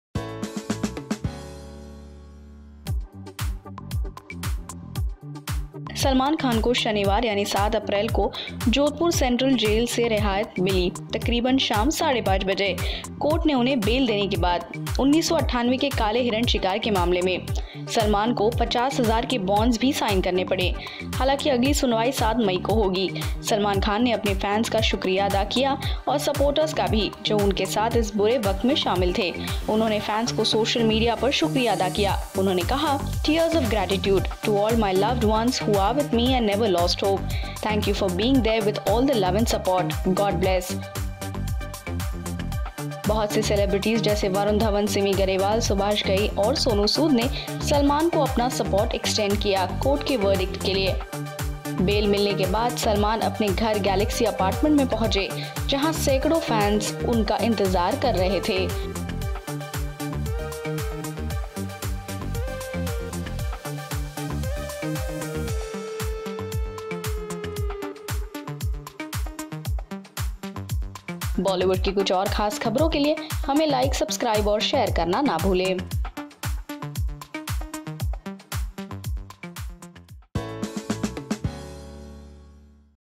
Salman Khan Thanks His Fans After His Release From Jail (כתוביות בעברית)
'Salman Khan on Saturday, April 7, walked out of the Jodhpur central jail at around 5.30 pmafter the court granted him bail in the 1998 blackbuck poaching case. He was asked to furnish two bonds of Rs 25,000 each.

While the next hearing is slated to May 7, Salman Khan has thanked his fans and supporters for standing by him during the tough time. He thanked his fans on social media site saying

"Tears of gratitude. To all my loved ones who are with me and never lost hope. Thank you for being there with all the love and support . God Bless."

Many celebrities like Varun Dhawan, Simi Garewal, Subhash Ghai and Sonu Sood extended their support for the actor during the court's verdict. After getting bail, Salman reached homeSaturday evening and waved to his fans and followers who had gathered outside his Galaxy apartment in Mumbai in large numbers.